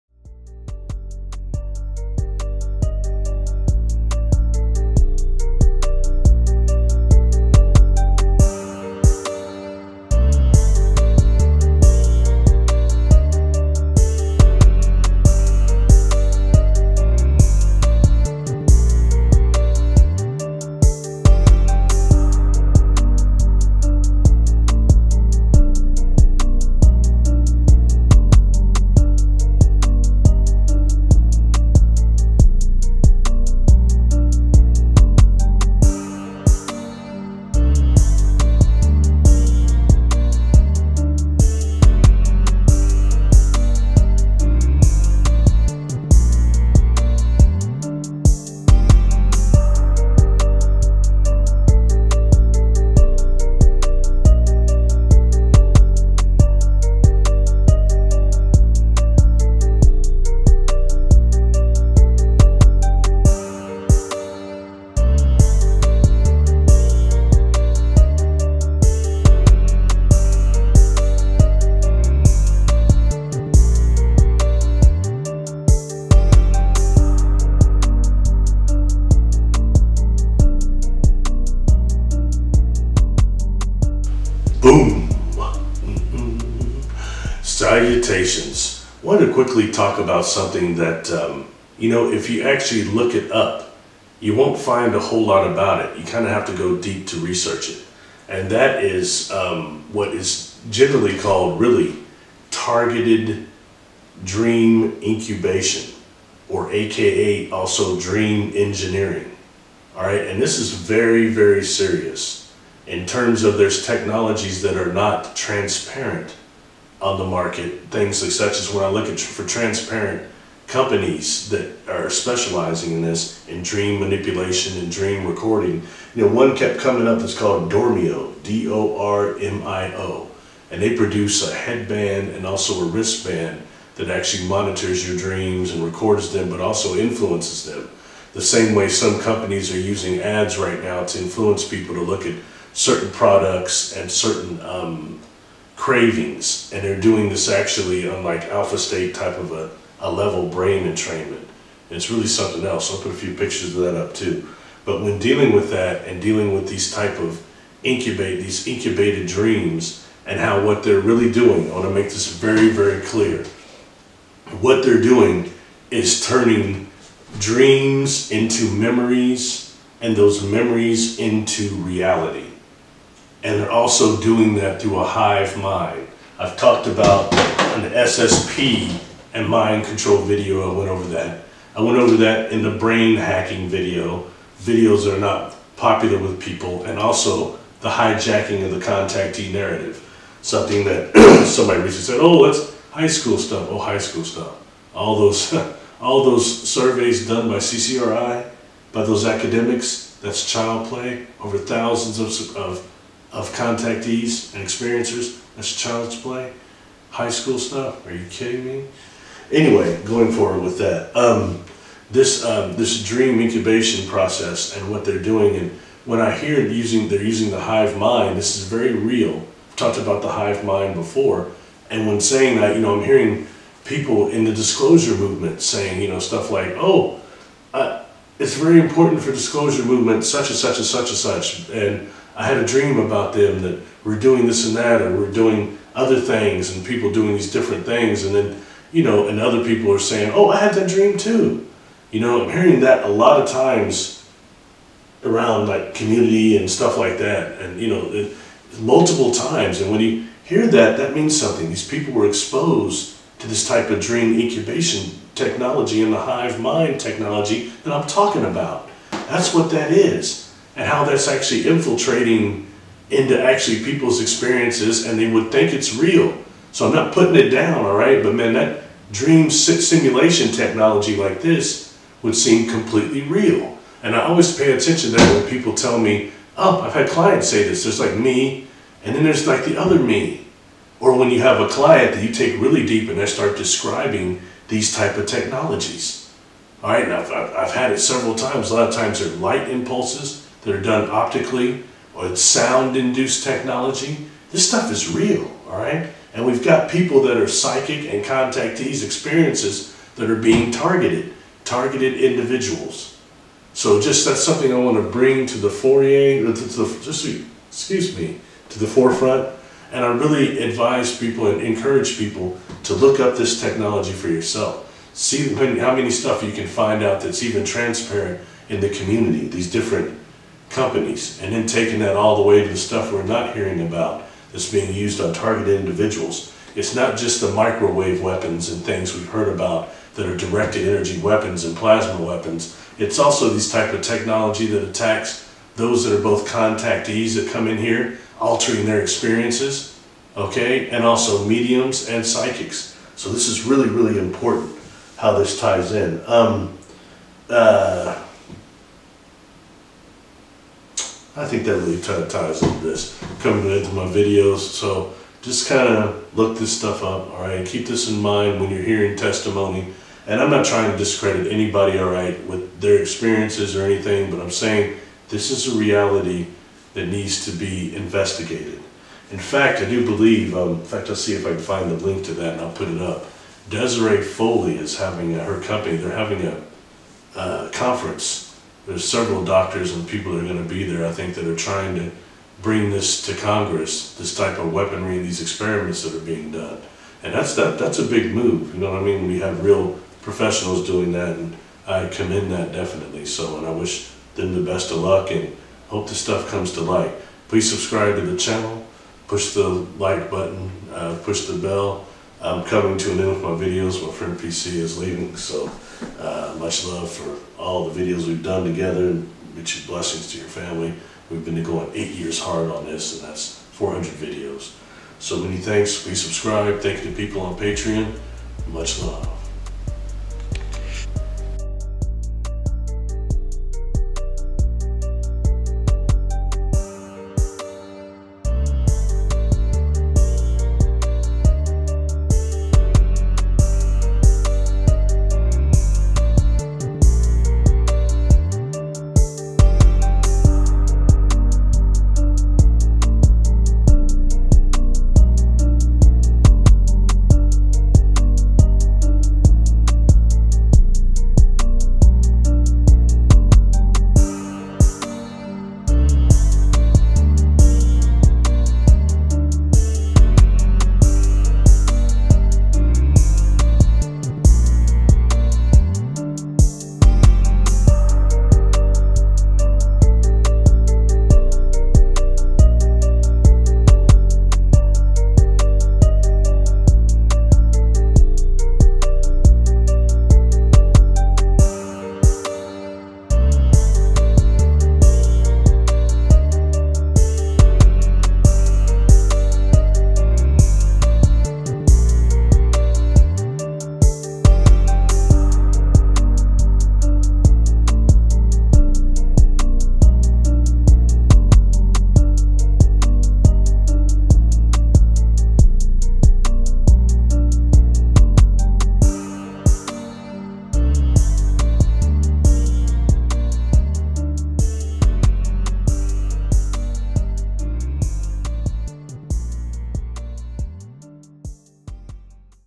Targeted Dream Incubation
Discussing the advanced & secretive field of "Engineering Dreams" through advanced technologies that can control, manipulate, & record the Dreamscape of unsuspecting people. Technology that turns "Dreams Into Memories" & those "Memories Into Reality".